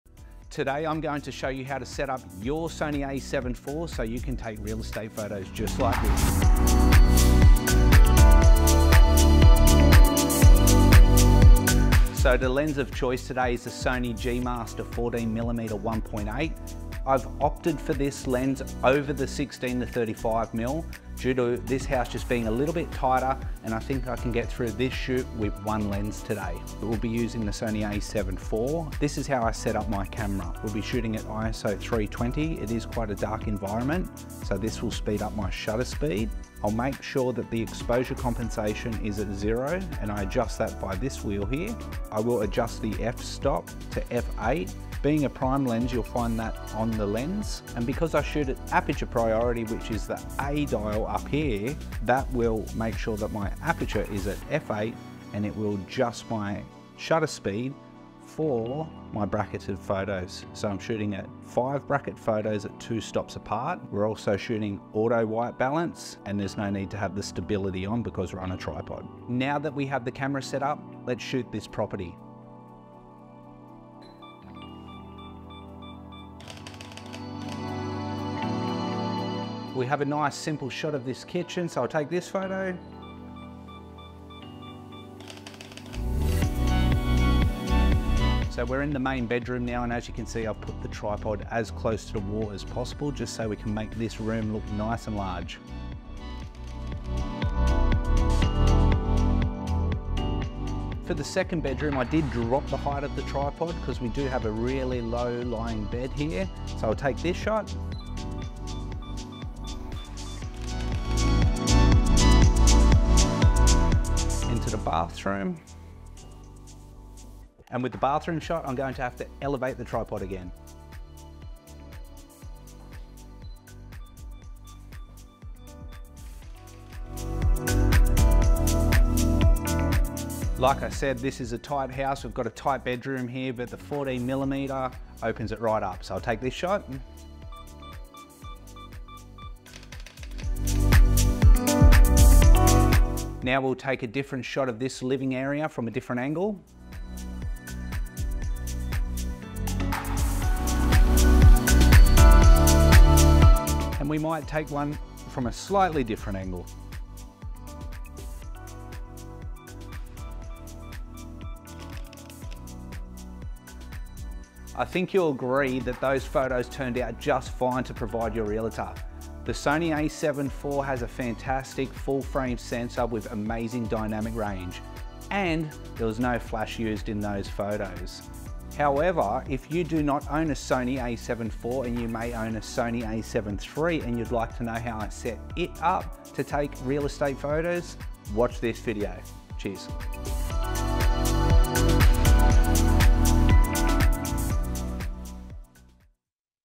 Real Estate Photography for Beginners _ Sony A7iv Settings
Are you a beginner in real estate photography? Look no further! This tutorial will show you how to set up your Sony a7 IV for capturing stunning property images. We'll explain why the Sony G Master 14mm 1.8 lens is the top choice for shooting real estate and guide you through the camera settings.